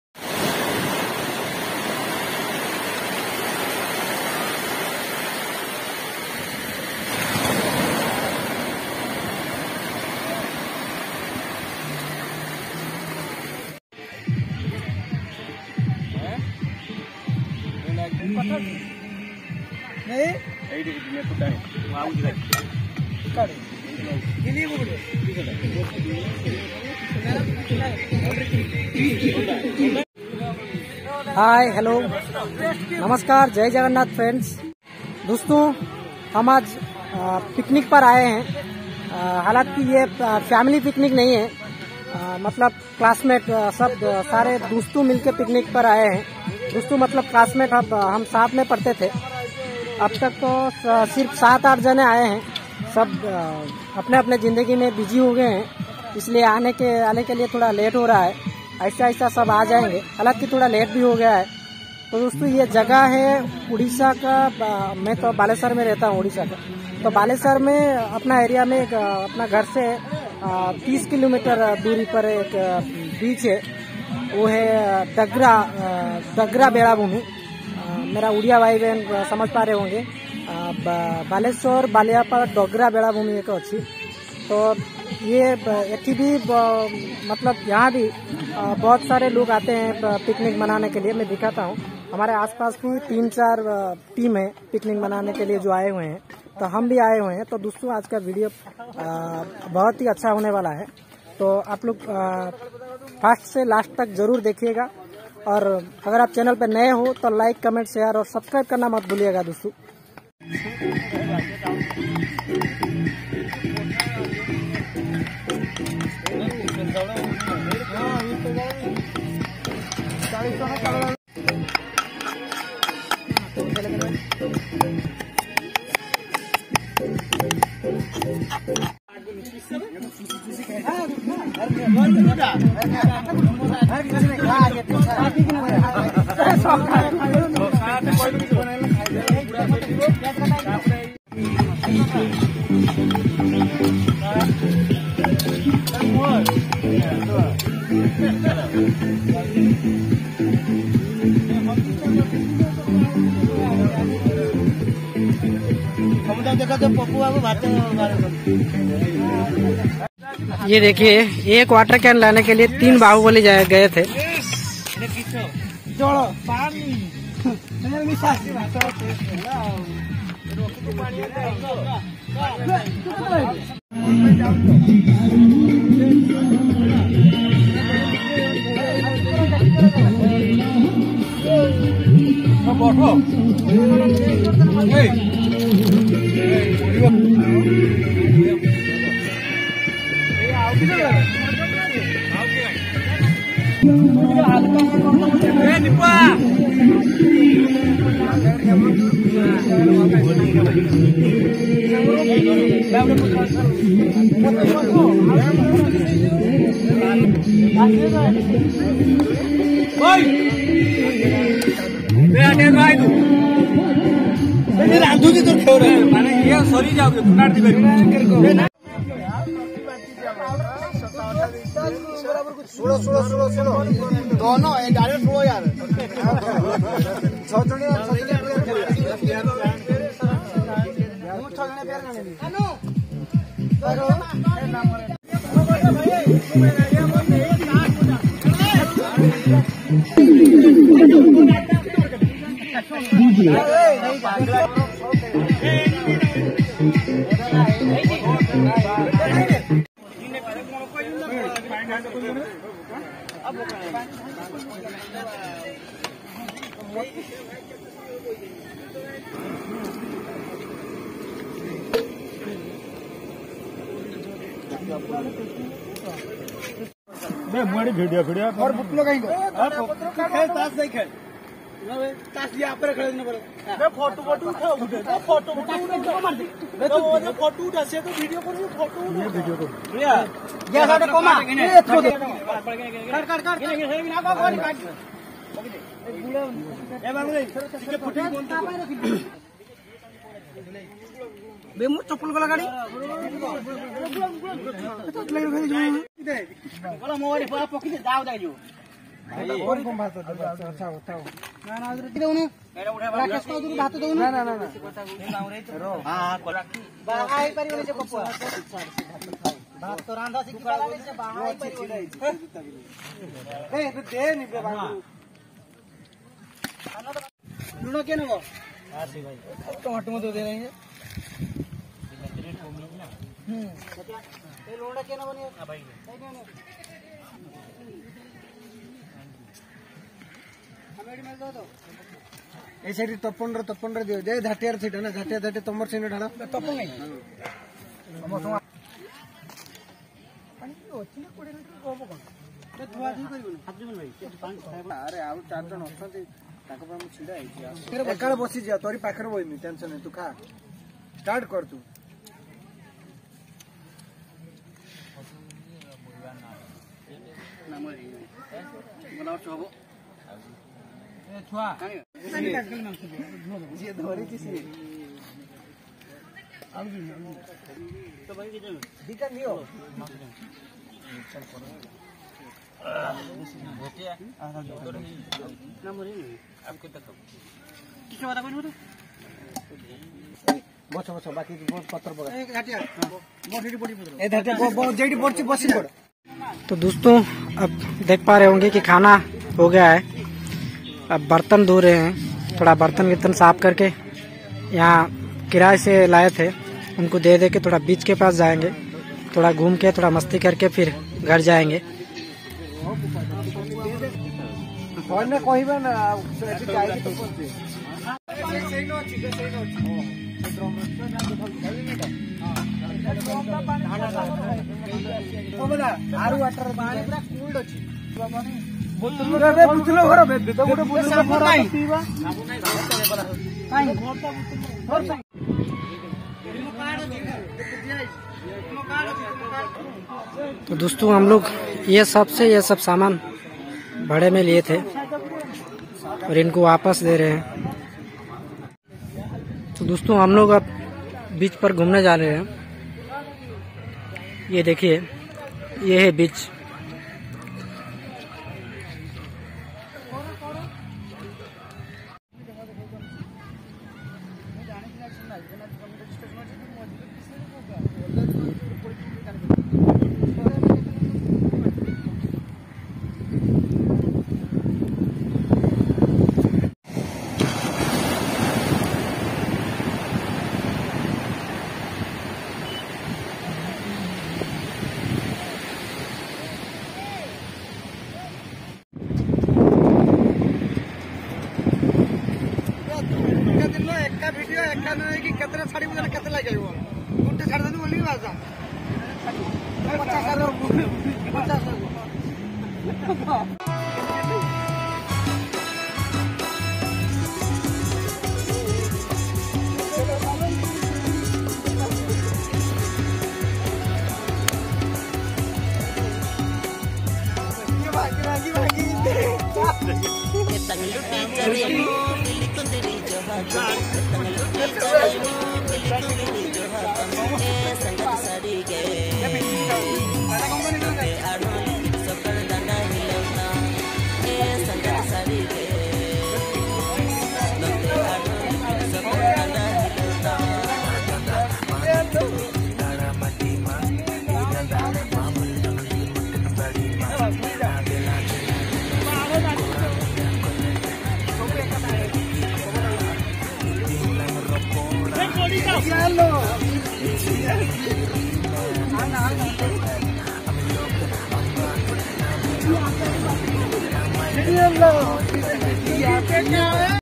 picnic rice chicken party | in desi village style.
main ek saadhaaran insaan hoon.laka laka eating show channel ke maadhyam se mera life style aapke saath sheyar kar raha hoon.
aaj main frends ke sath picnic kiya hun.